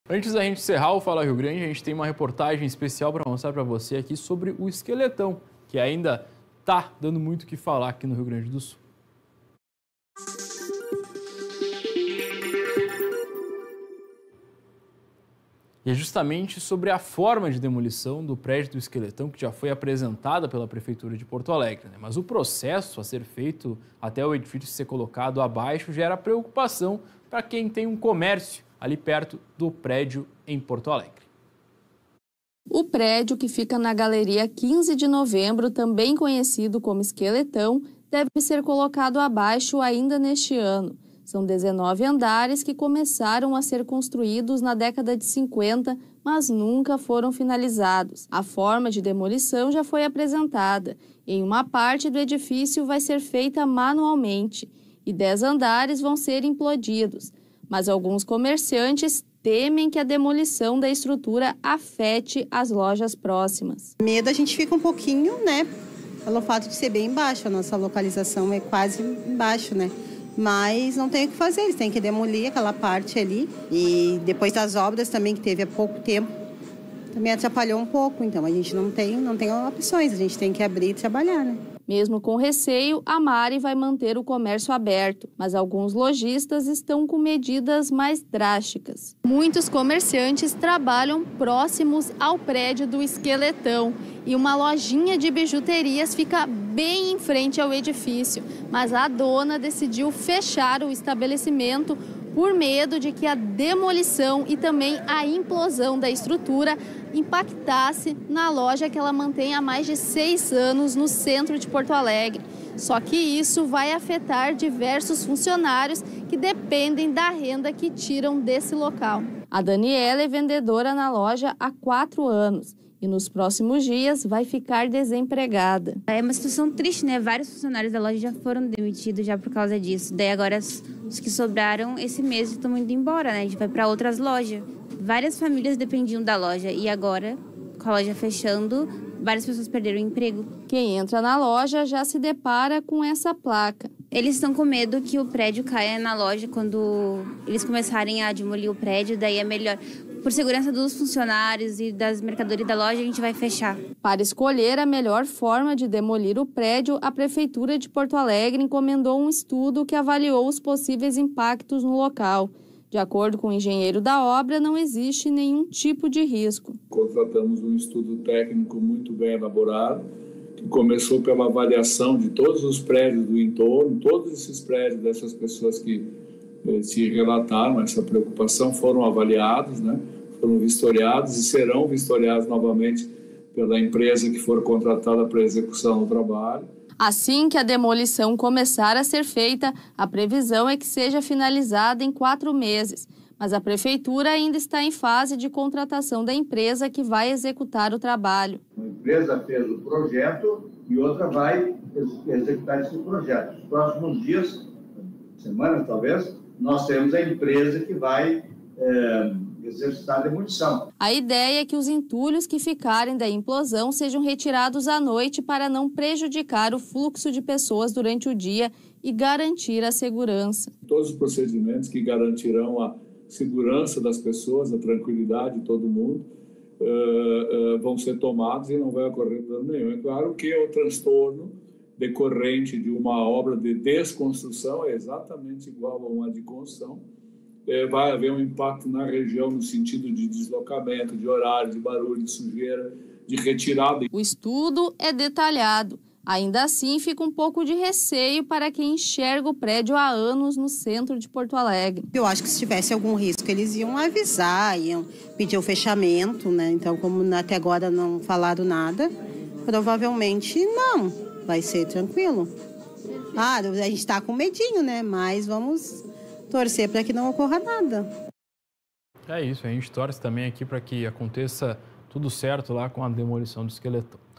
Comerciantes temem a demolição do esqueletão | Fala Rio Grande 17/08/2023
Processo de demolição preocupa donos de comércios próximos.